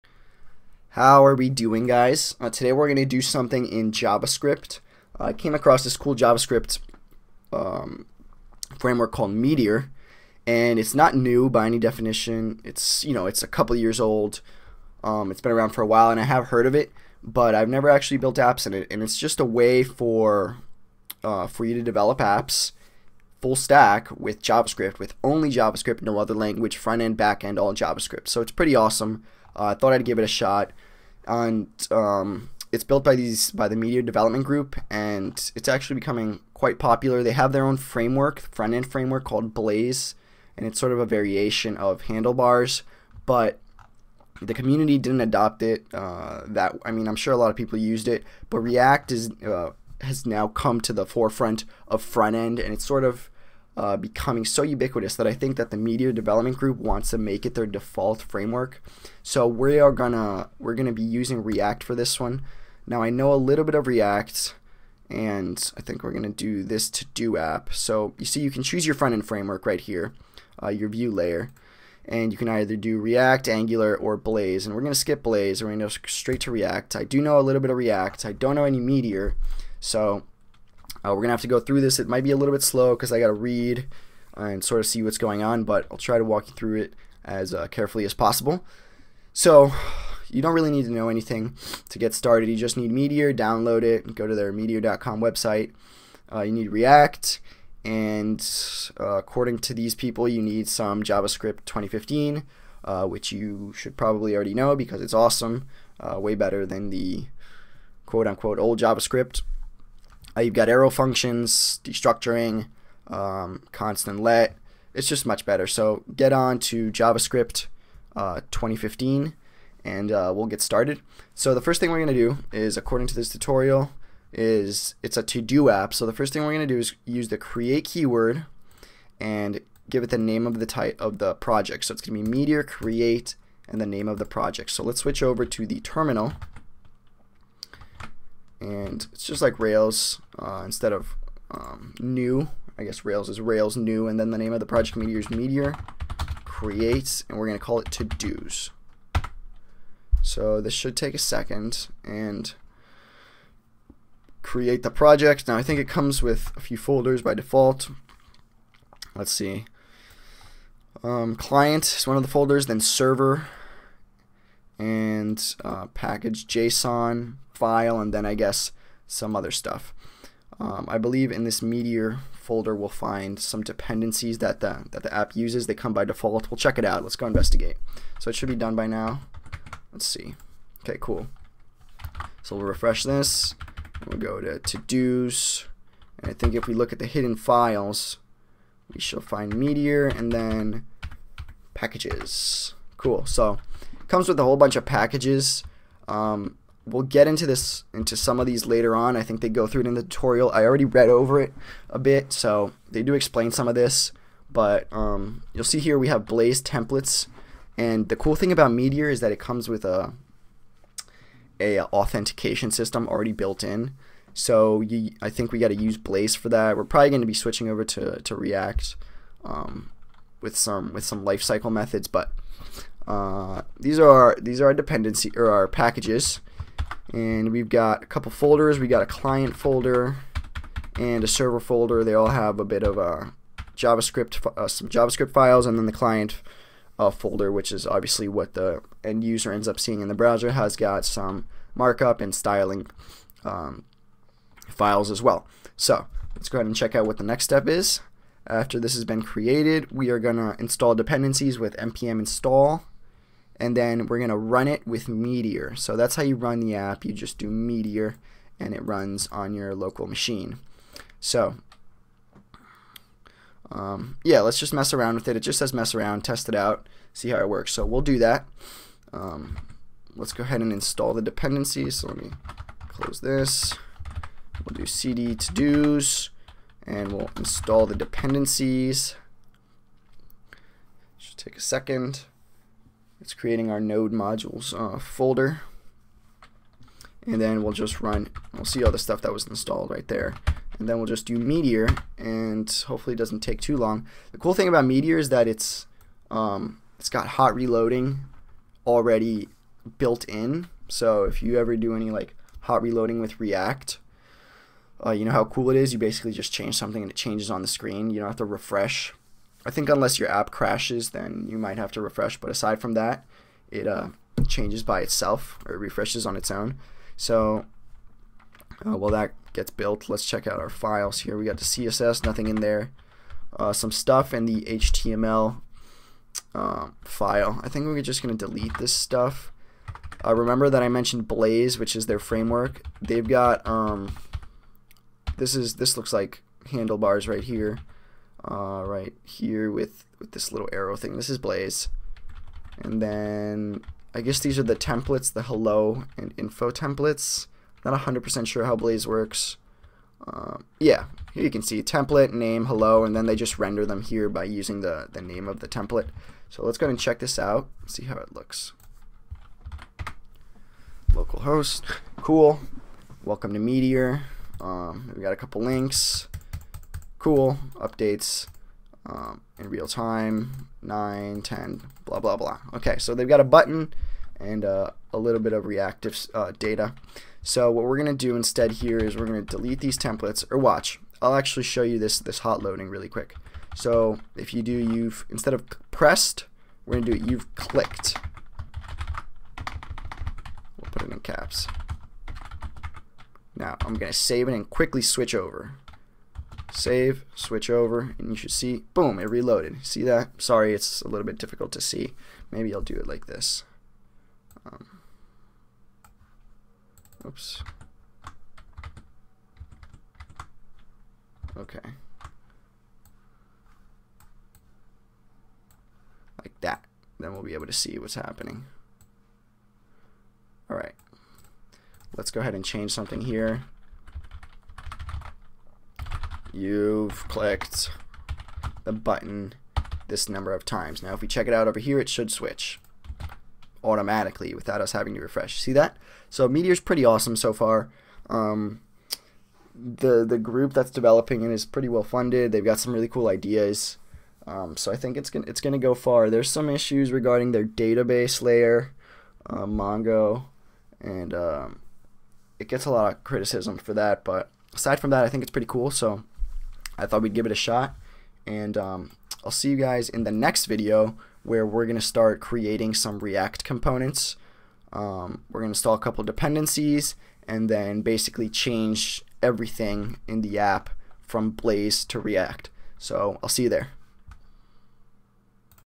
Meteor + React To Do App
I'm trying to learn Meteor so I did the tutorial on meteor.com and walk through it in this series.